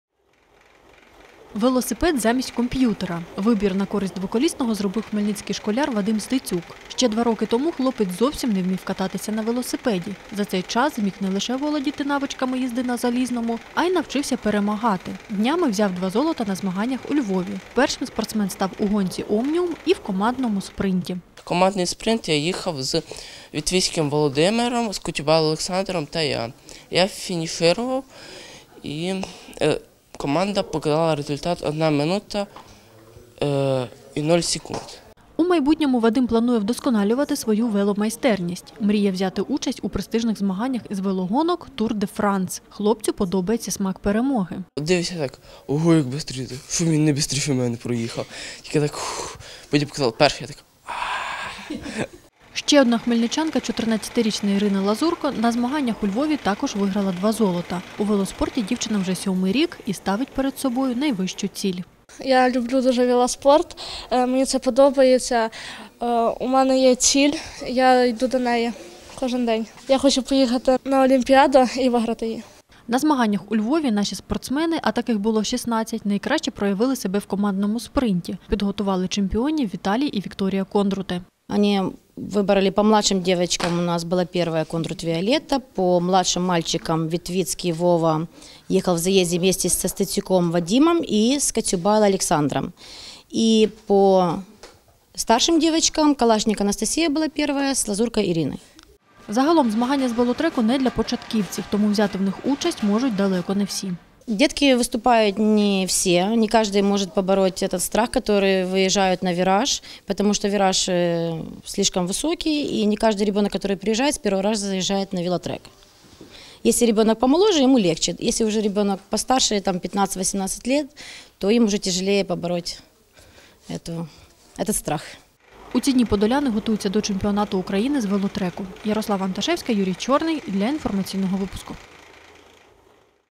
Хмельничани привезли 18 медалей зі змагань з велотреку у Львові
На швидкості не менше, ніж 25 км за годину – саме настільки треба розігнатися спортсмену, щоб втриматися на велотреку. Вихованцям Хмельницької дитячо-юнацької спортивної школи це вдалося. З відкритих обласних змагань у Львові наші спортсмени привезли 18 медалей, 10 з яких – золоті.